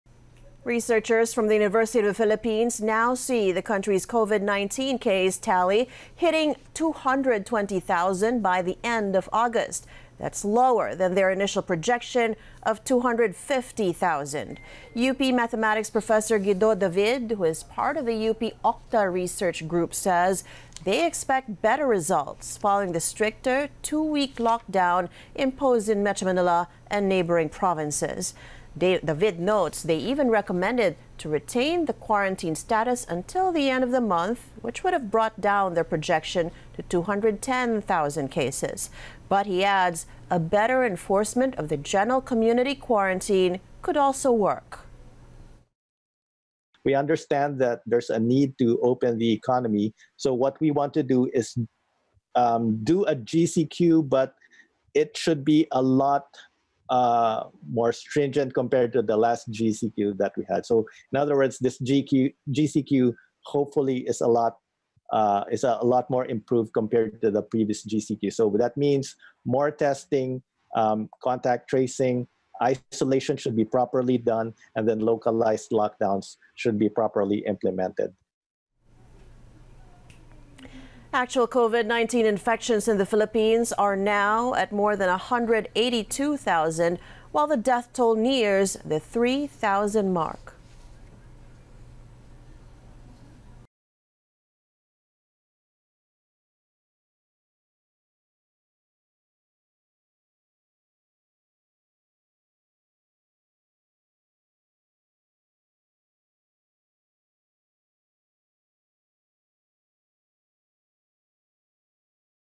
U.P. researchers lower August projection of COVID-19 cases to 220,000 | ANC
Researchers from the University of the Philippines now see the country's COVID-19 case tally hitting 220,000 by the end of August, lower than their initial projection of 250,000.